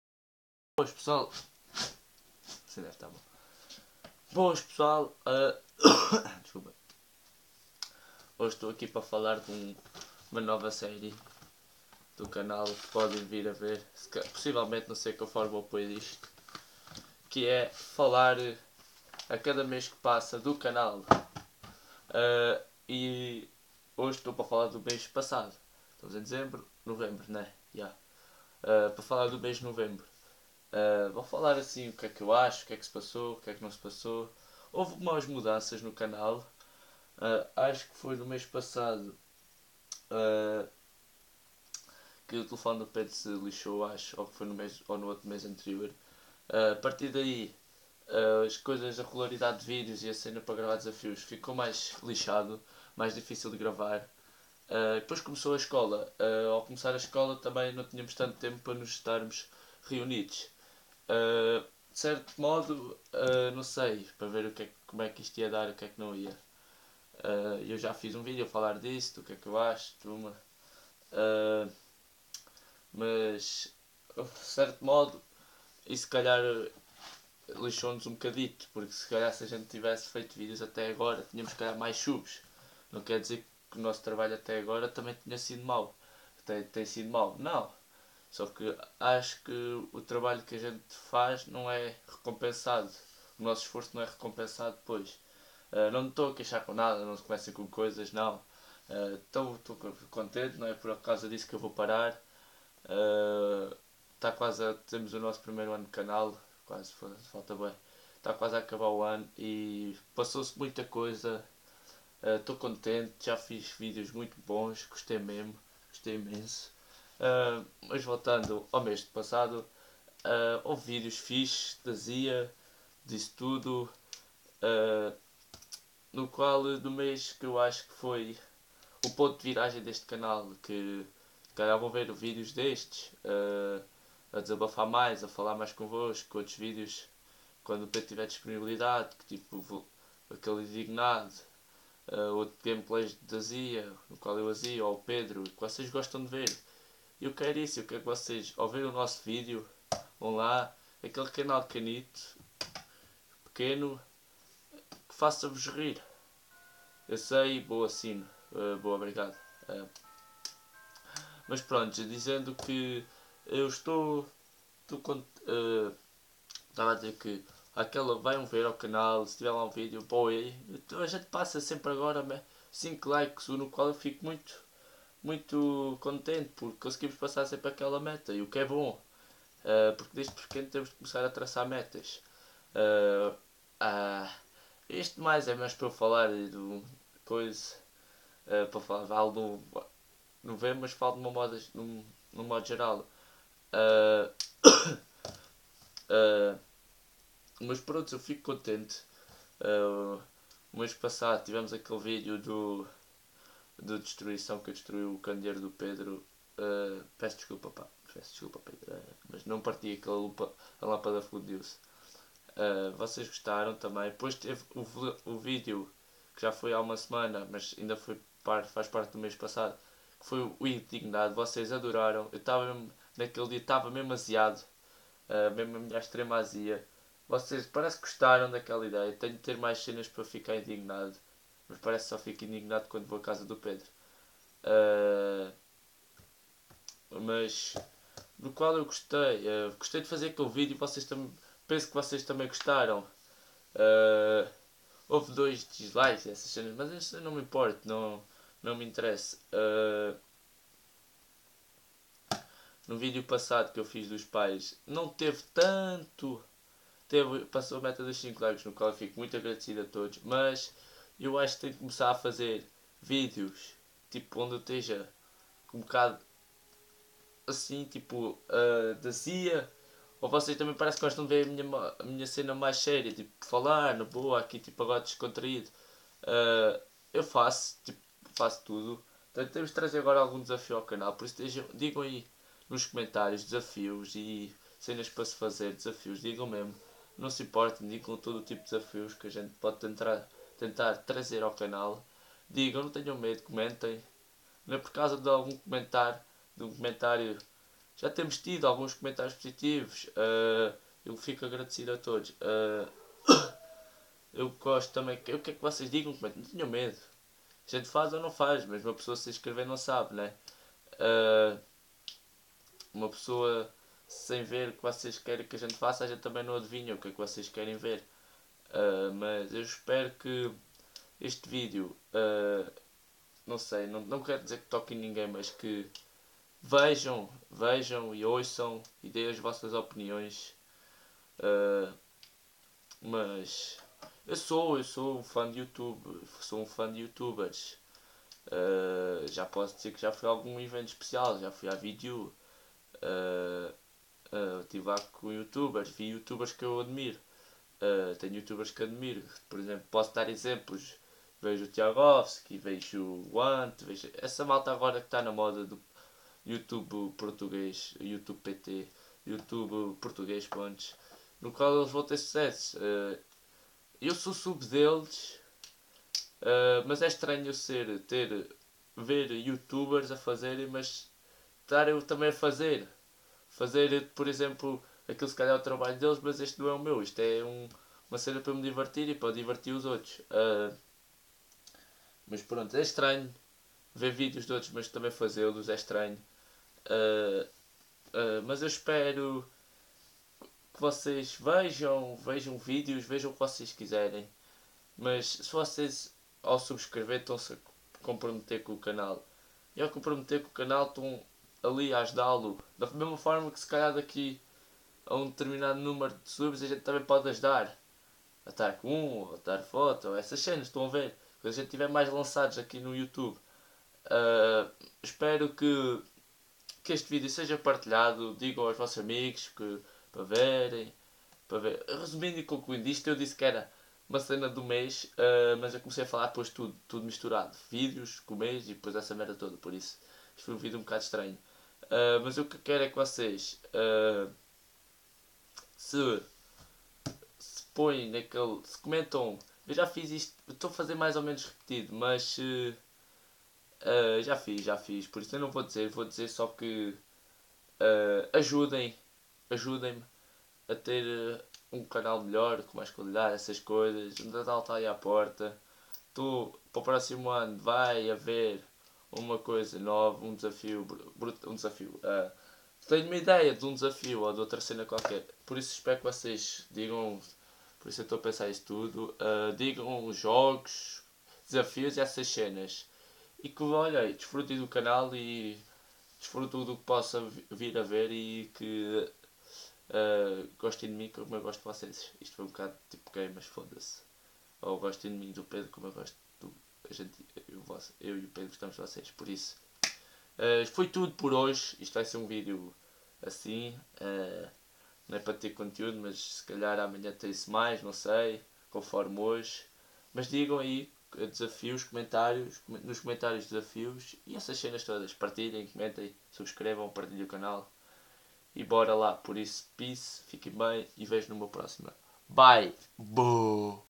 Outra Serie no Canal?? Digam-me vocês....
Manos mais um video no qual tive a desabafar,falar com vocês.
Espero que partilhem o video, comentem e deixei o vosso gosto e é tudo por agora.
E fuiiiiiiiii!!!